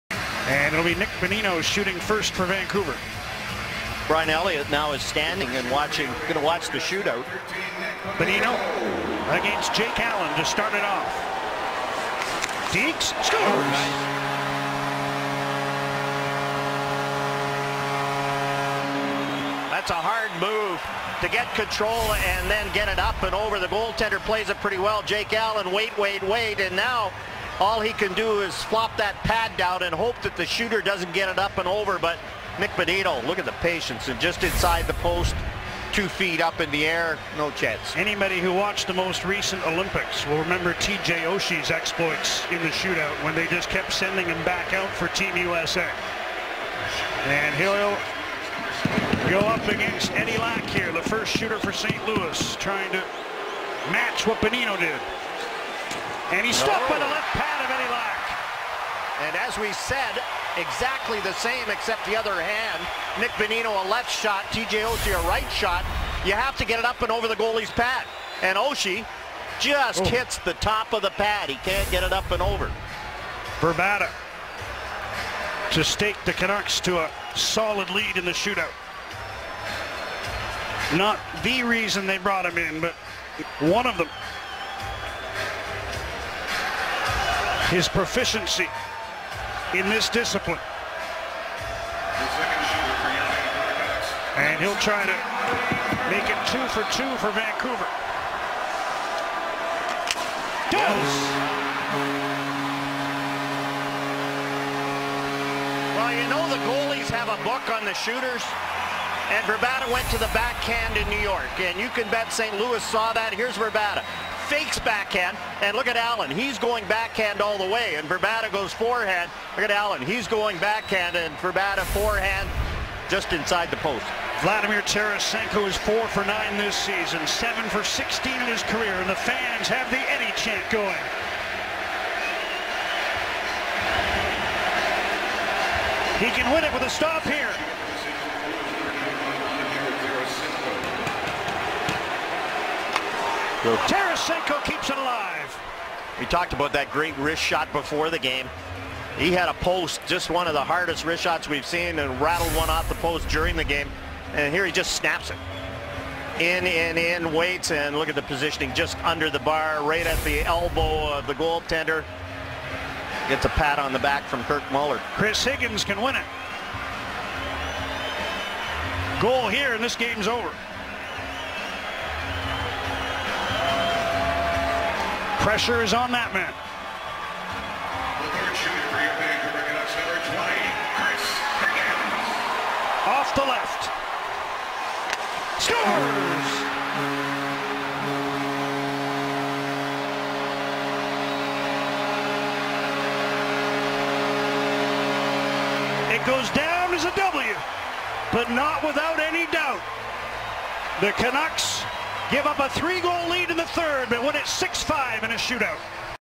Canucks vs Blues - Shootout - 3/1/15 - HD
Vancouver Canucks vs St Louis Blues. Shootout. March 1st 2015.

Footage from Sportsnet

Property of the NHL